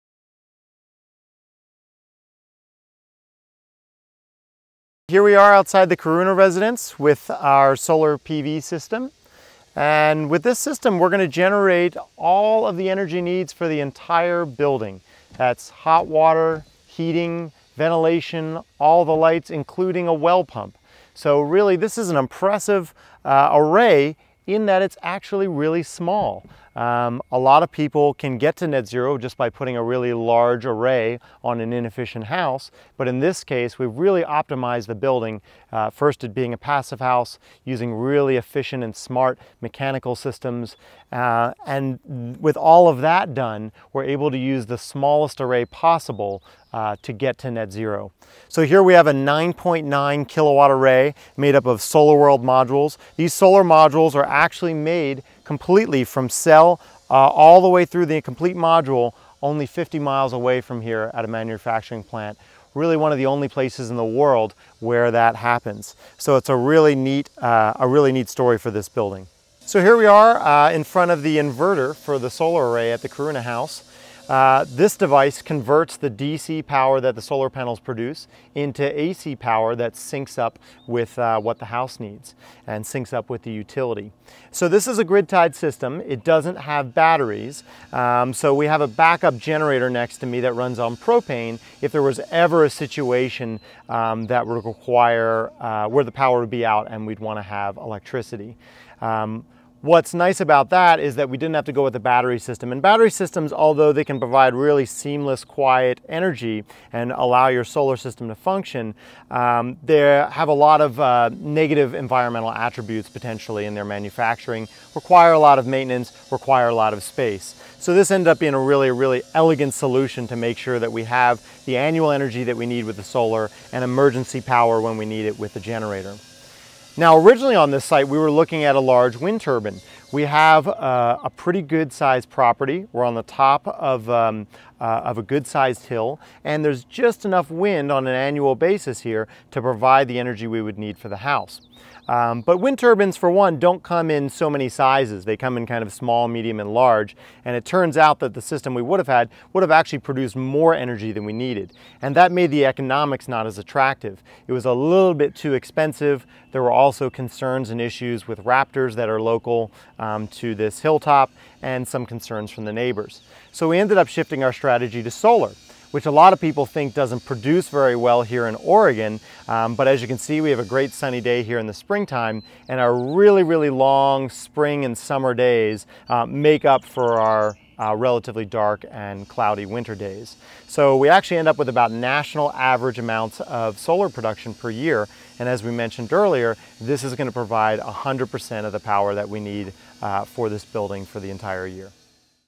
Karuna Passive House Solar PV System
Owner of Imagine Energy, Jonathan Cohen details the solar PV system installed at the Karuna Passive House.